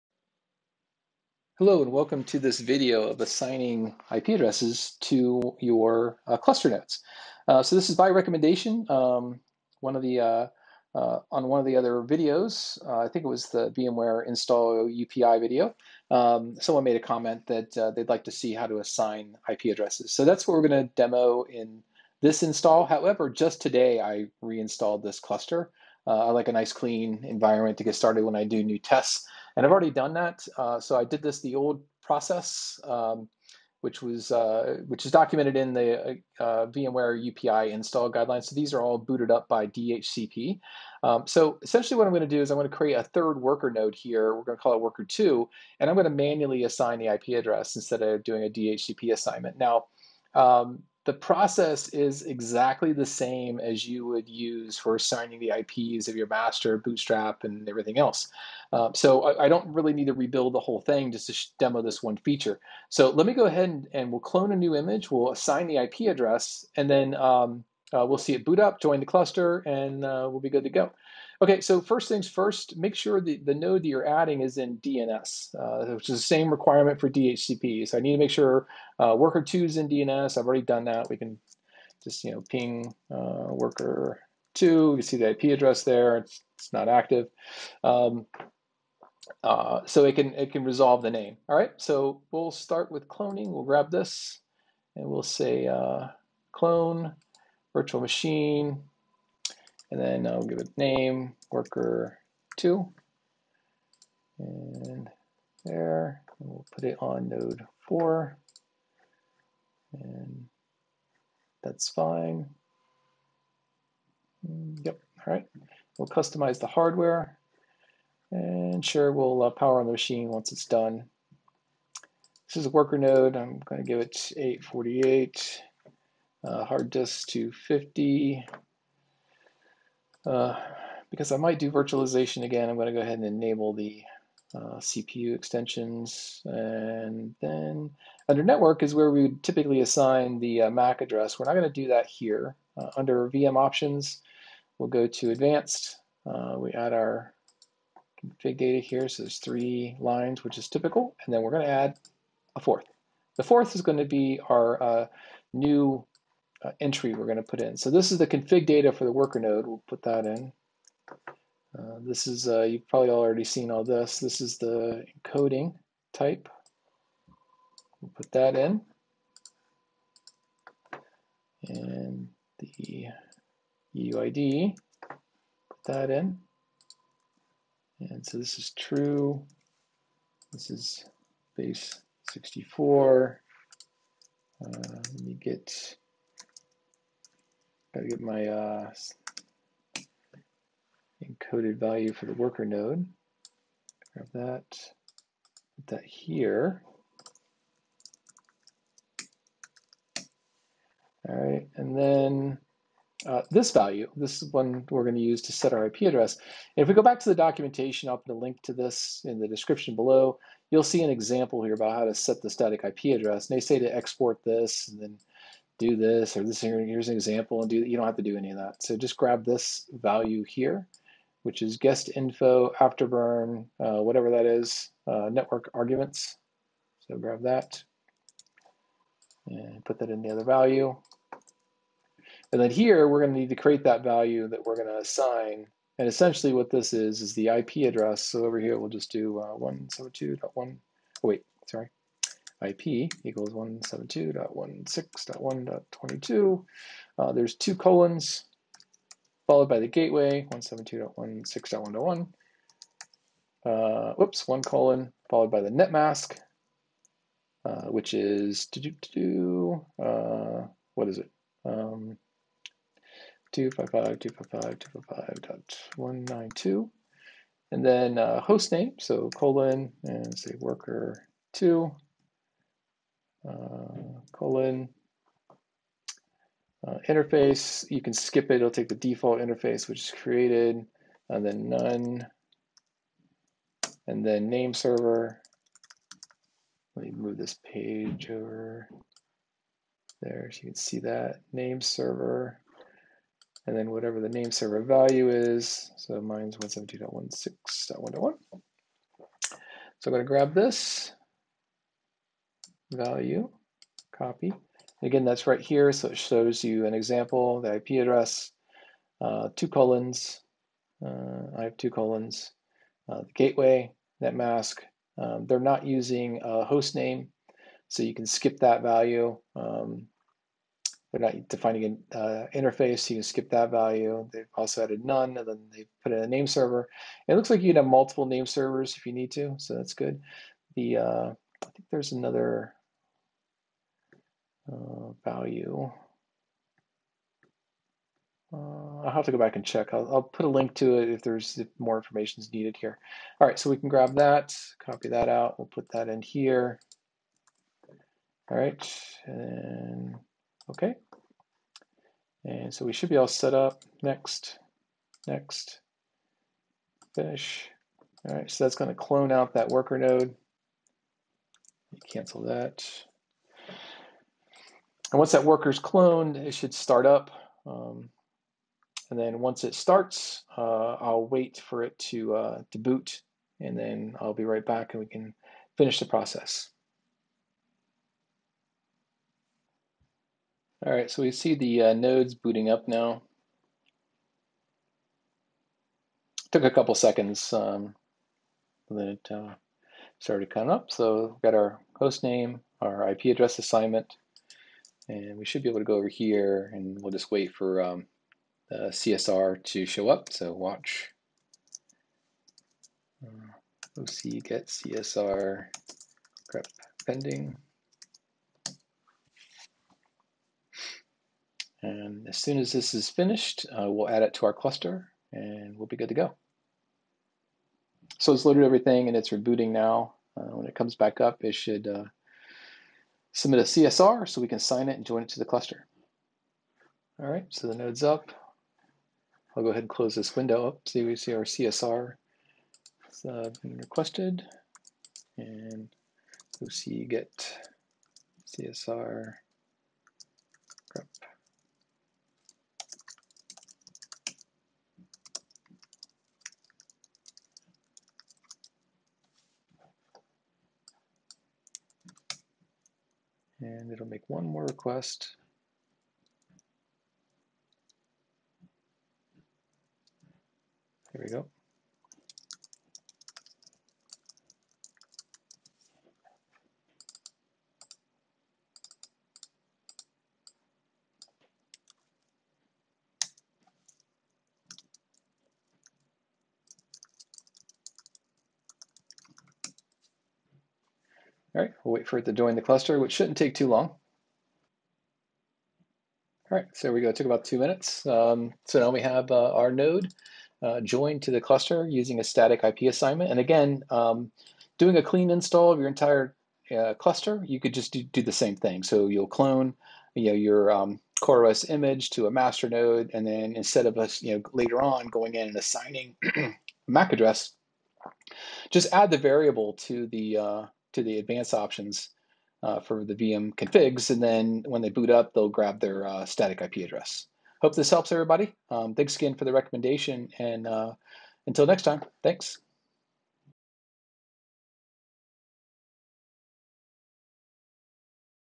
OpenShift VMware UPI Install w/ Static IP Addresses
In this brief tutorial, I demonstrate adding a node to my cluster by "static IP", this procedure is identical to building the cluster initially simply by adding the new values to Advanced VM Options.
key = "guestinfo.afterburn.initrd.network-kargs"
example value = "ip= /ip/::/gateway/:/netmask/:/hostname/:/iface/:none nameserver=srv1 [nameserver=srv2 [nameserver=srv3 [...]]]"